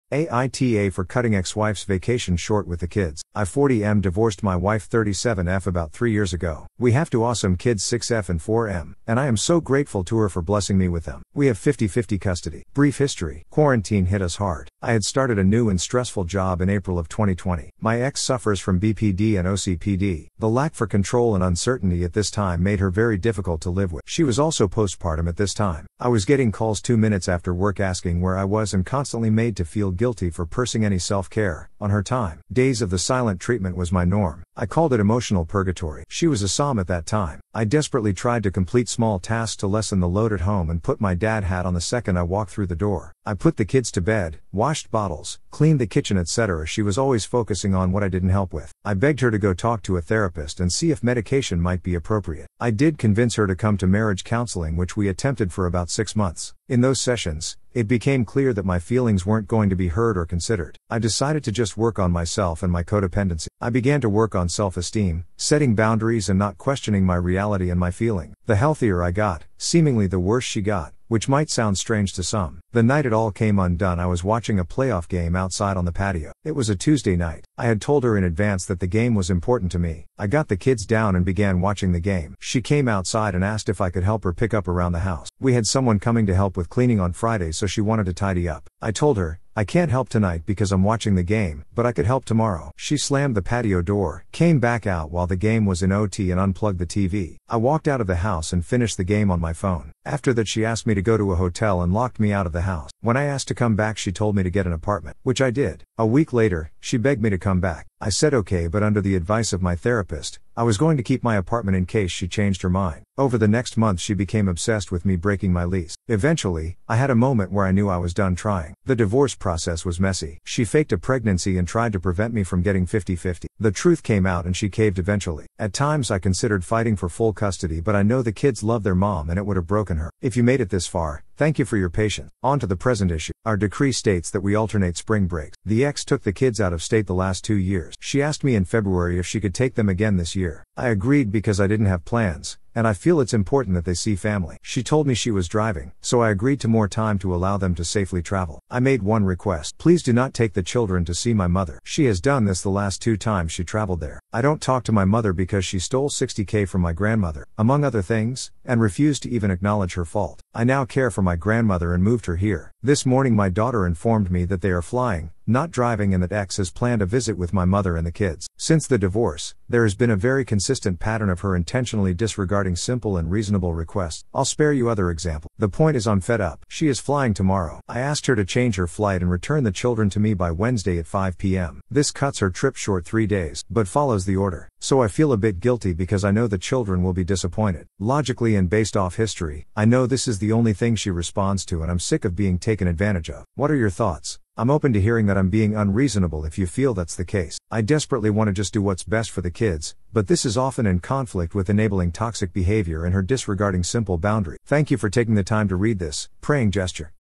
AITAH for Cutting ex-wife’s vacation short with the kids.
AITAH for cutting my ex-wife’s vacation short with our kids due to her disregarding my request not to take them to see my mother? Despite guilt, I acted to enforce boundaries and address consistent disregard for reasonable requests.